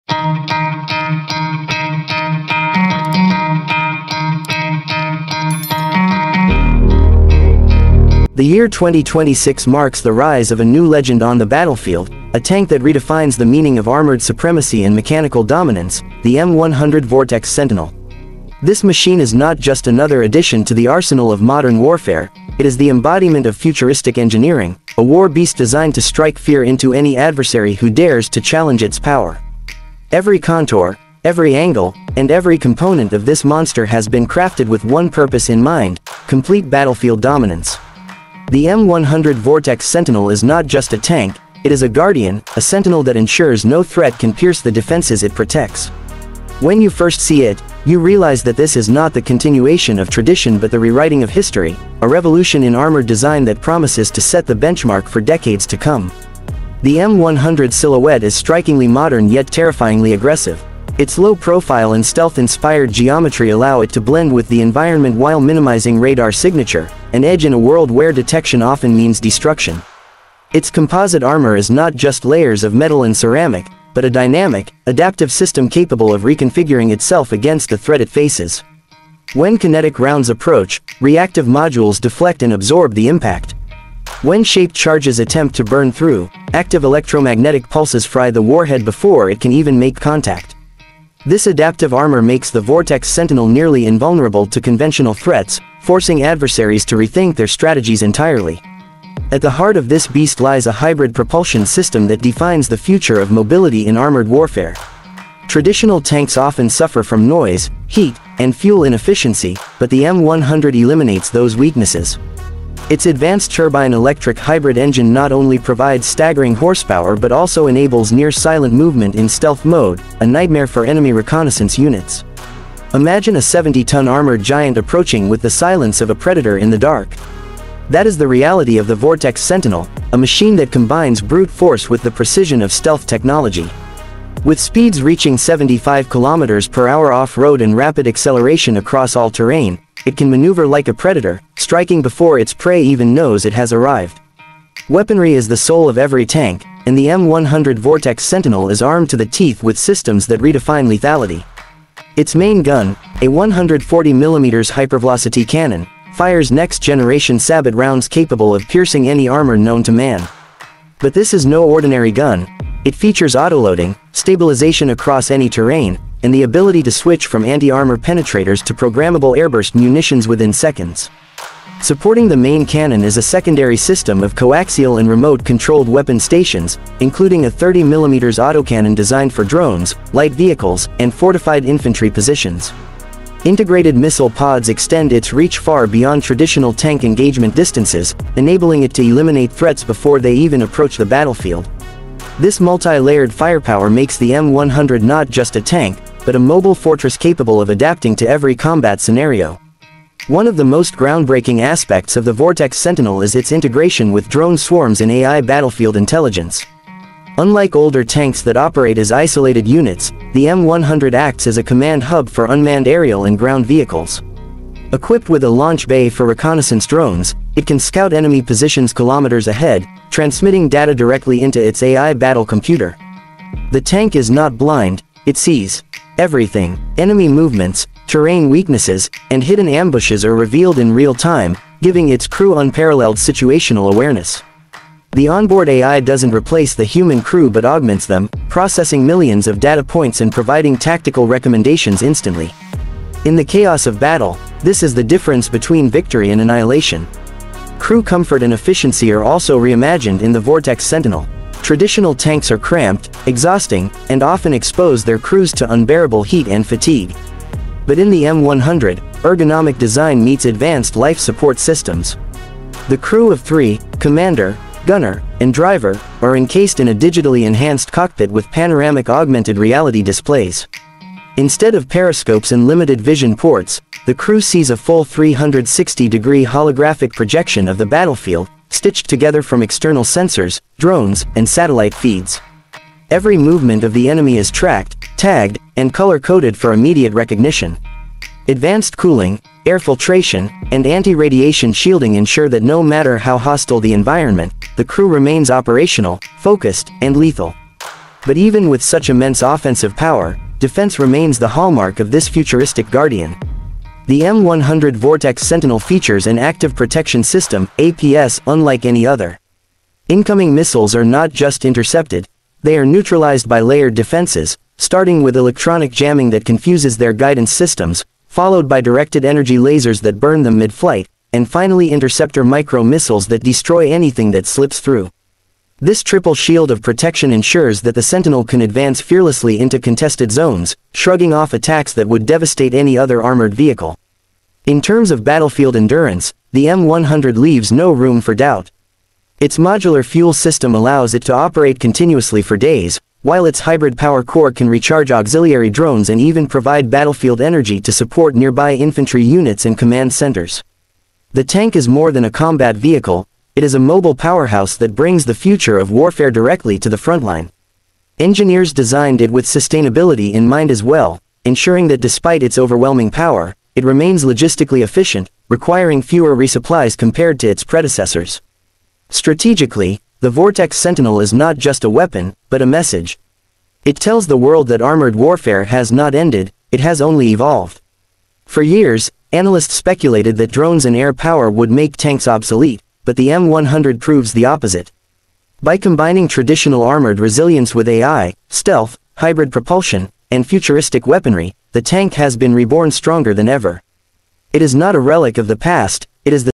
“M100 Vortex Sentinel 2026 – The Tank That Changed Modern Warfare Forever”
description.

The M100 Vortex Sentinel 2026 is not just a tank—it’s a revolution. Designed with stealth geometry, adaptive armor, a hybrid propulsion system, and AI-powered battlefield intelligence, this war machine redefines dominance on the battlefield. Armed with a 140mm hypervelocity cannon, drone integration, and triple-layered active protection, the Sentinel is a futuristic guardian built for absolute supremacy. From deserts to urban jungles, it adapts, survives, and destroys with unmatched precision. This is the future of armored warfare, and you’re about to witness history in motion.